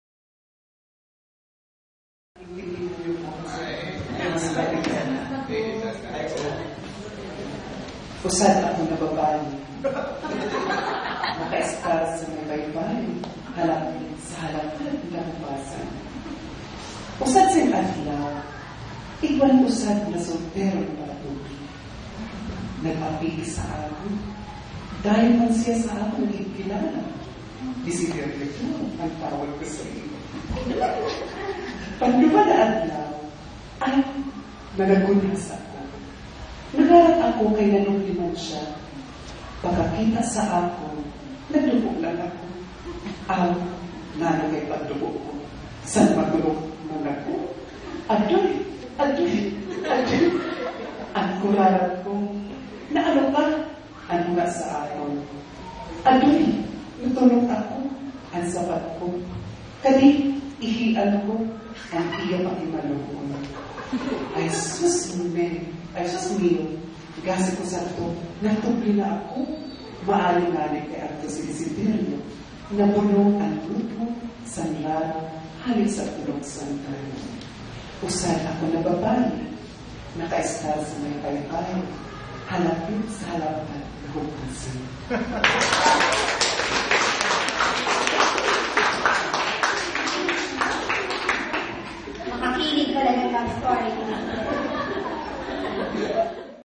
Roda Añosa -- Bicol Poet
Rodel Anosa reading Desiderio